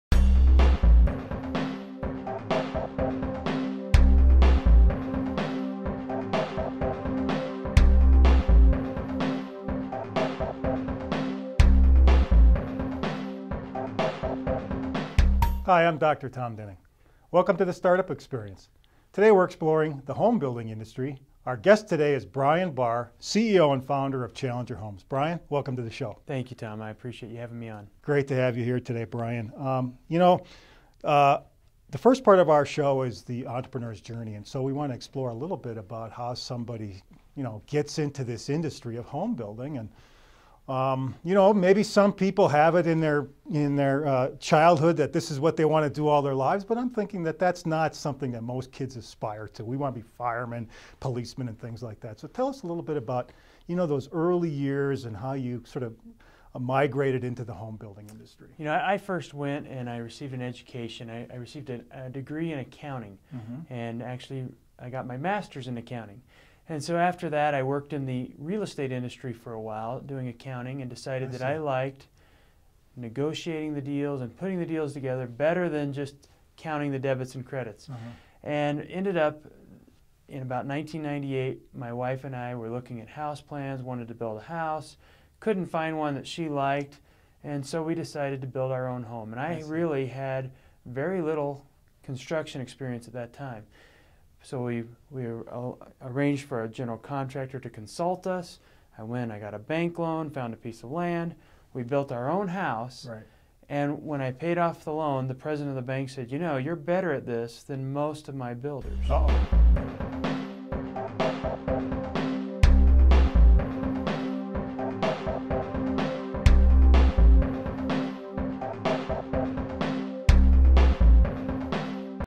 Start a Home Building Company
Want to start a home building company? Learn from someone who has succeeded in the business. Entrepreneurship expert Dr. Tom Duening talks with Brian Bahr, founder of Challenger Homes, who has been a professional home builder for 11 years.  Brian has grown his business into one of the top five building firms in his state. In the hourlong, on-demand video interview available Brian offers detailed advice on how he would start a home building company today

Find out how to finance each home project
Get insider tips on how to leverage buyers
Gain insight into designing a floor plan
Learn to build a team of reliable trade partners
Hear how to find a niche and dominate the market